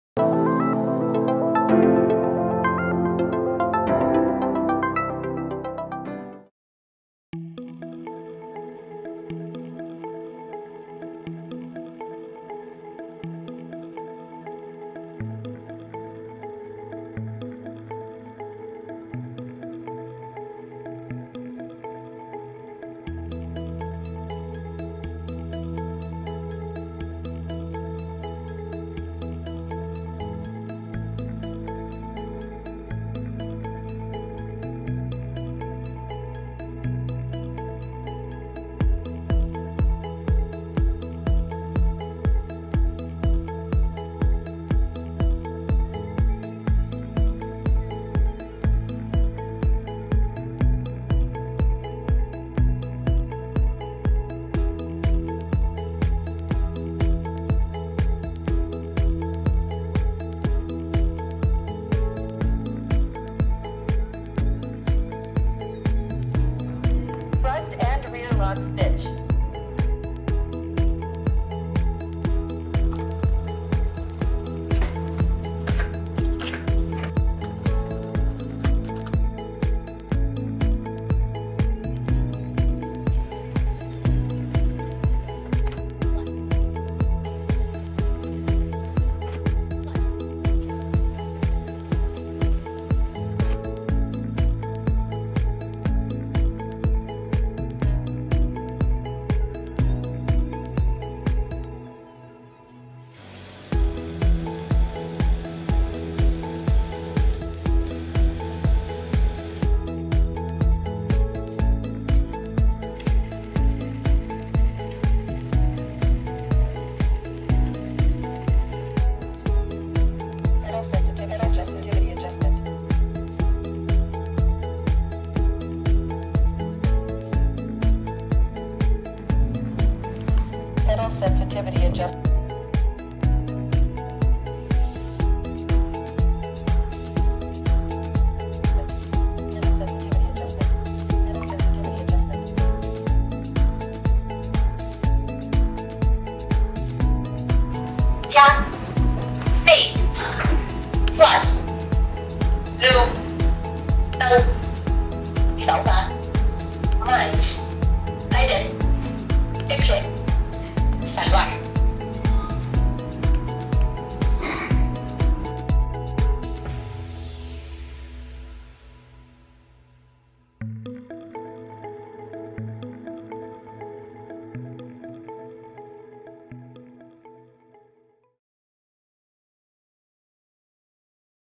MCZJA7100 Intro (Full Option Machine)
The MCZJA7100 is a flagship model from Zoje, one of China's leading brands in the machinery industry. This full-option machine is renowned for its advanced features, precision, and durability, making it a preferred choice for professionals in various sectors. With its state-of-the-art technology and comprehensive functionality, the MCZJA7100 ensures high performance and efficiency, catering to the diverse needs of modern manufacturing processes.

Direct Drive Technology
Upto 70% power saving
Auto Thread Trimmer
Auto Reverse
Auto Presser Foot Lifter
Programmable Sewing
Low Noise Low Vibration
Speed Control
Needle Position Control
3 Level Adjustable LED Lamp
Caster Wheels